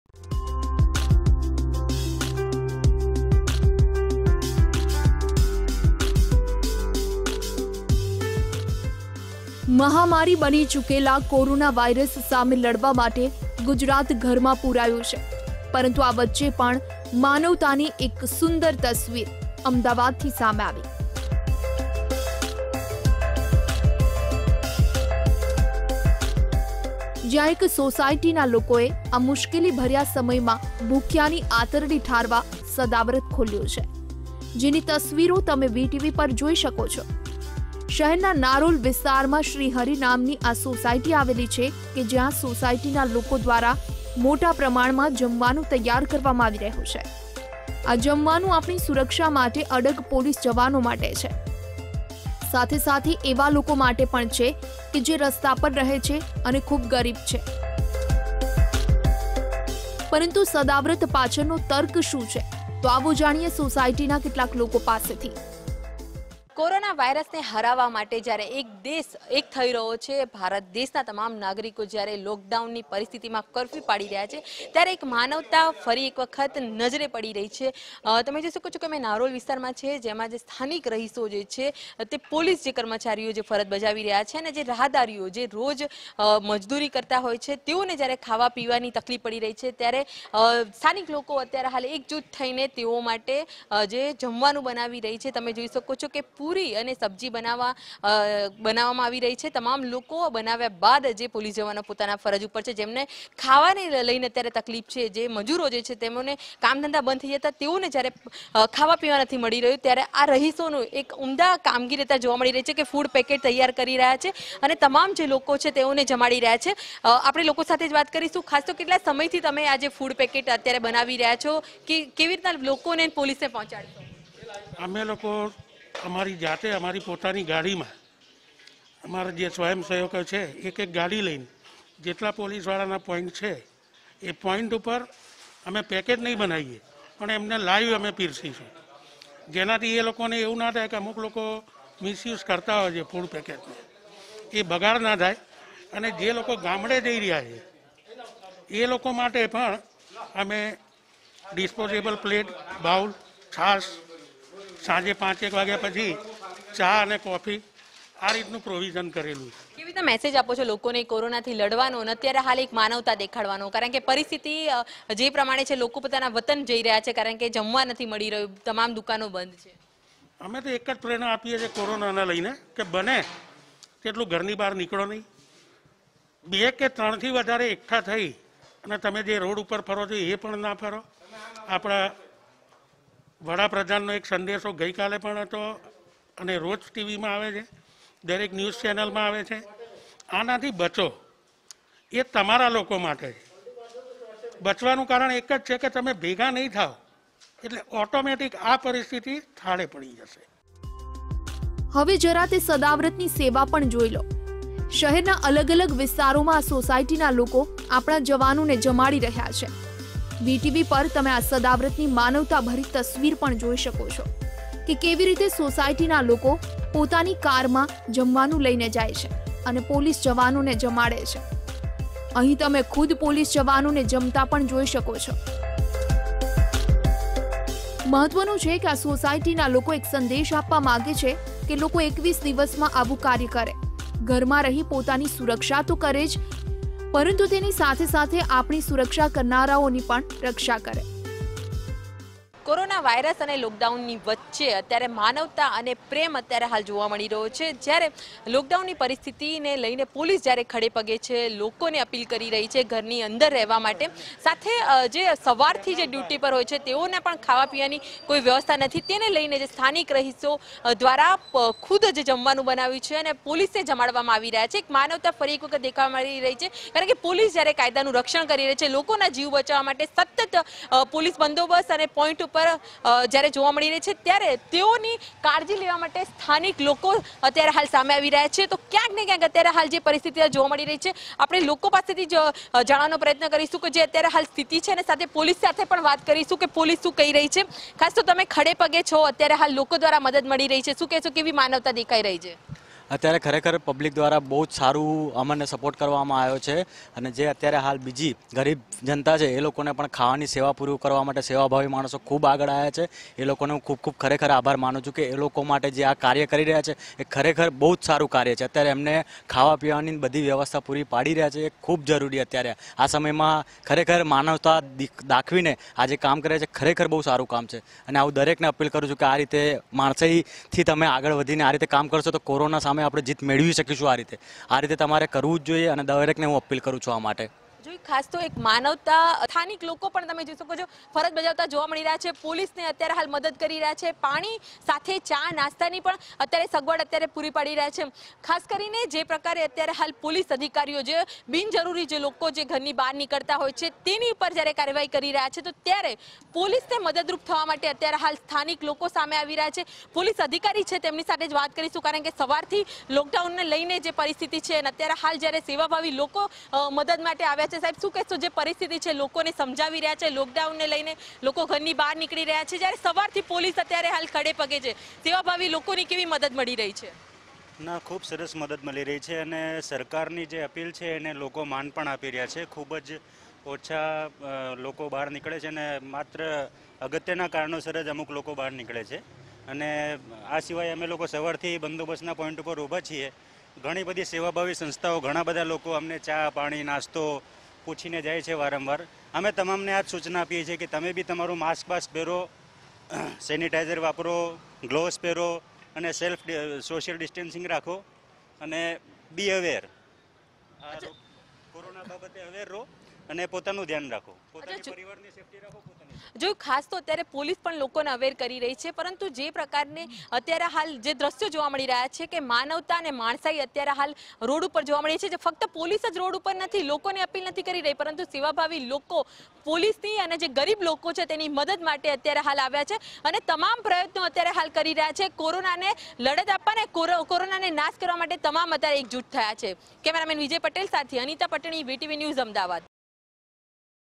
ગુજરાત બંધઃ એક એવી સોસાયટી જ્યાંના લોકોએ સ્વખર્ચે જમવાનું બનાવી જરૂરિયાત મંદો સુધી પહોંચાડ્યું | VTV
ગુજરાત બંધઃ એક એવી સોસાયટી જ્યાંના લોકોએ સ્વખર્ચે જમવાનું બનાવી જરૂરિયાત મંદો સુધી પહોંચાડ્યું